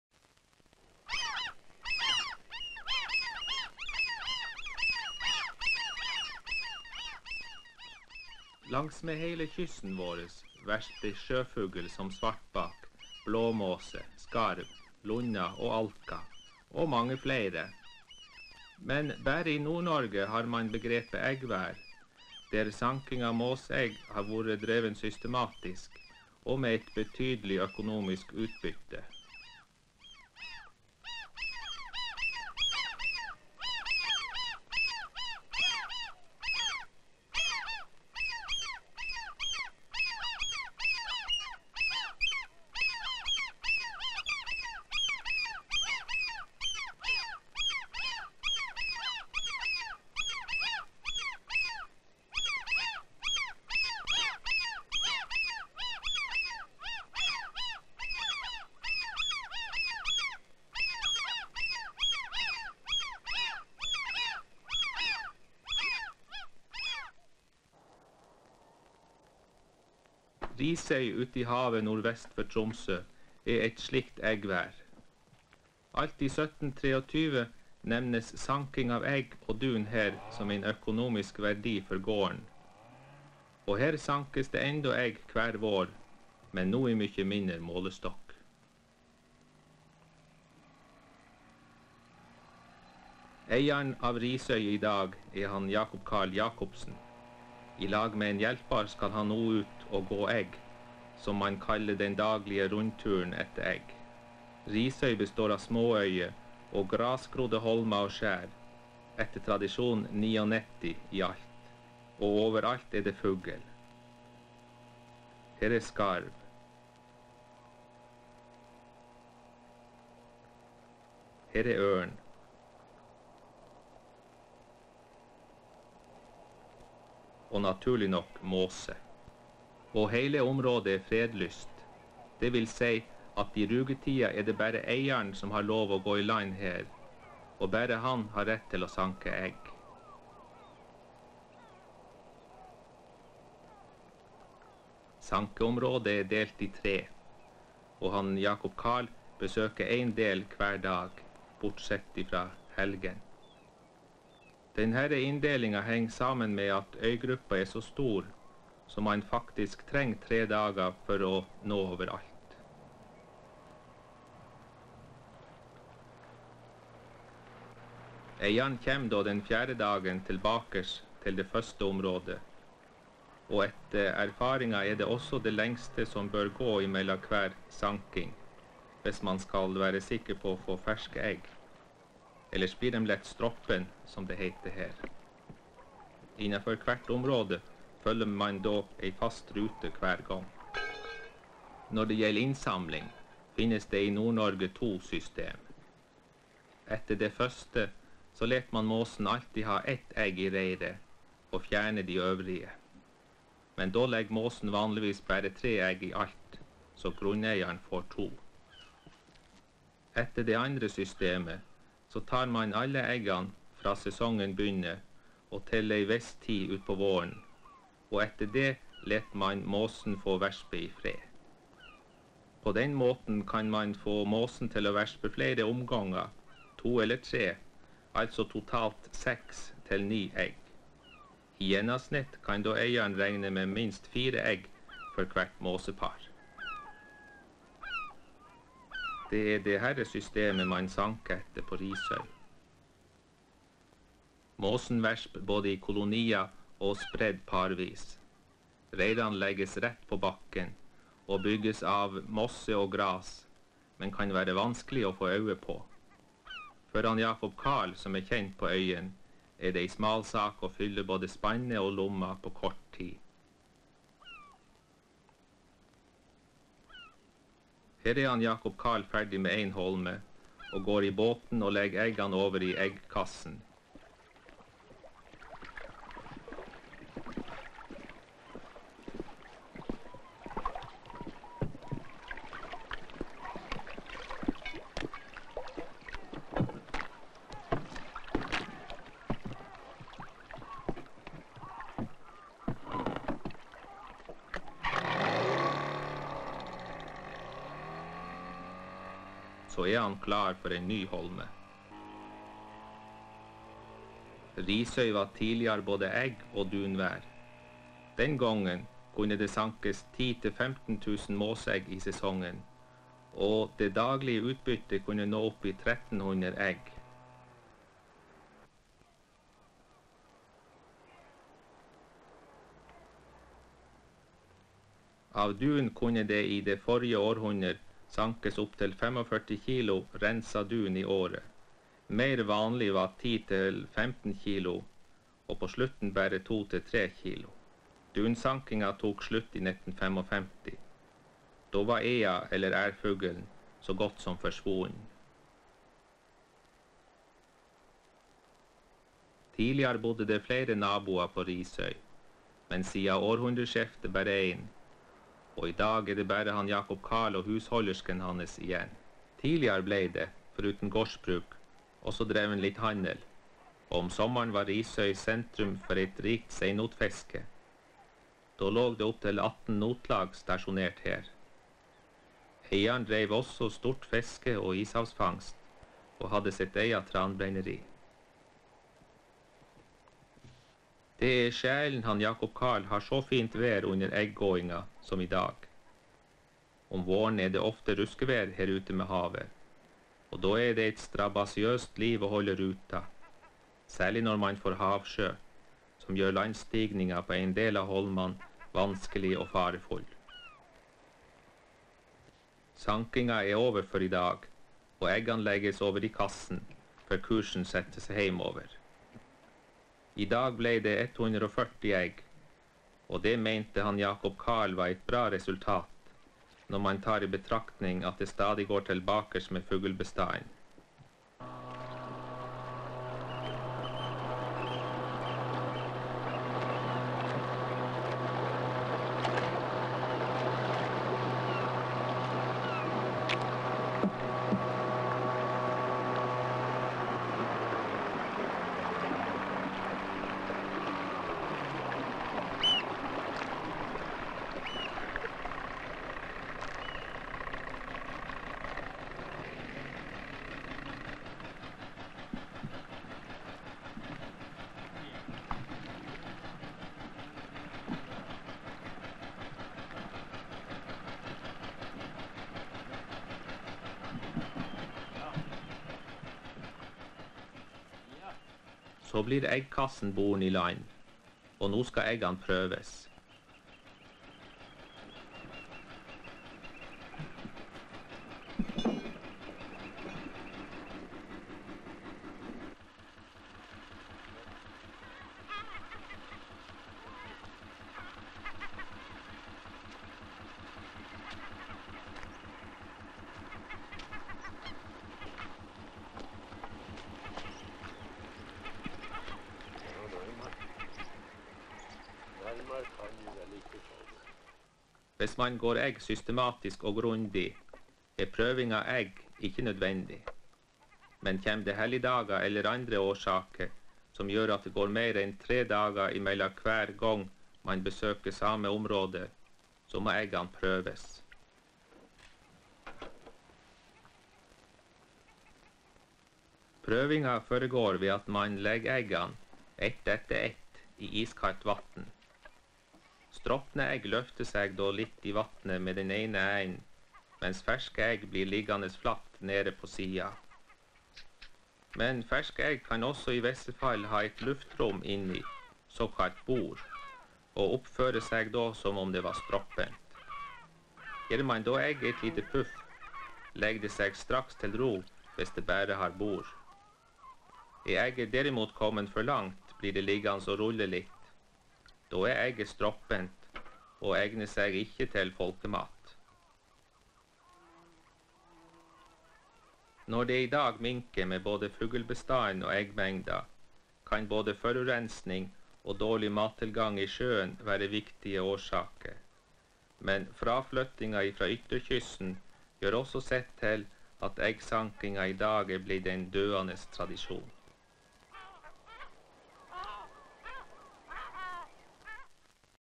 Eggvær : Risøy i Troms 1974
Sjøfugl hekker langs hele kysten, men bare i Nord-Norge har man begrepet "eggvær", der samling av måsegg har vært drevet systematisk og med betydelig økonomisk utbytte. Ulike former for "reking" av eggvær, "tredagersgåing" av eggvær og utsortering av "stropne" egg.
(fra SIFT)